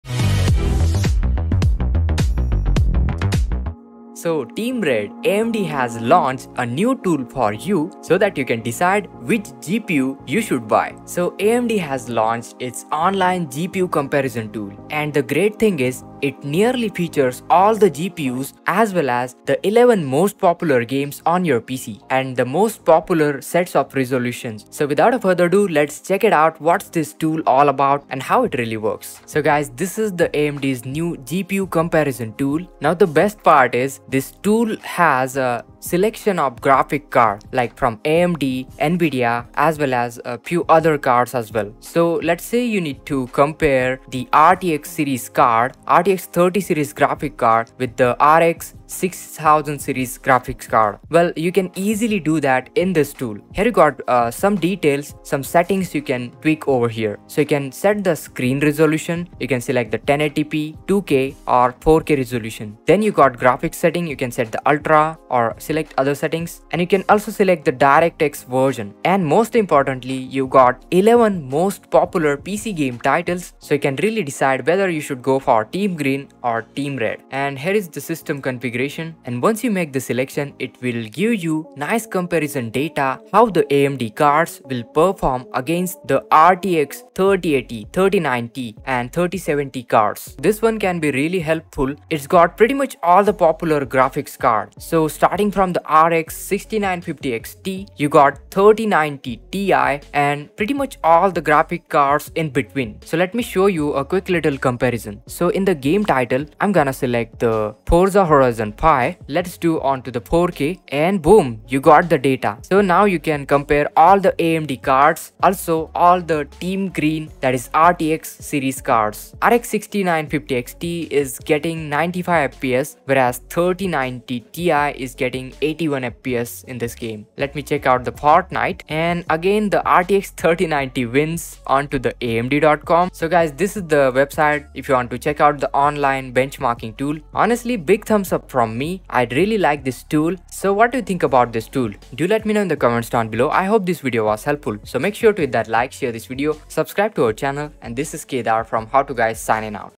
NVIDIA vs AMD Gaming Benchmark Graphics Card Tests 🎮 GPU Comparison Tool RTX VS RX
Compare NVIDIA VS AMD graphic cards RX 6950 XT, 6900 XT, 6800 XT, 6800 VS RTX 3090 Ti, 3080 Ti, 3090, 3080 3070 Ti at click of a button. Get Benchmark Graphic Card Test.
See the performance of AMD Radeon 6000 Series GPUs in the latest games and settings using the comparison tool below. For full transparency, a competitive comparison with the Nvidia RTX 3000 Series graphics cards is also provided. All performance numbers have been generated and verified by testing in AMD internal labs.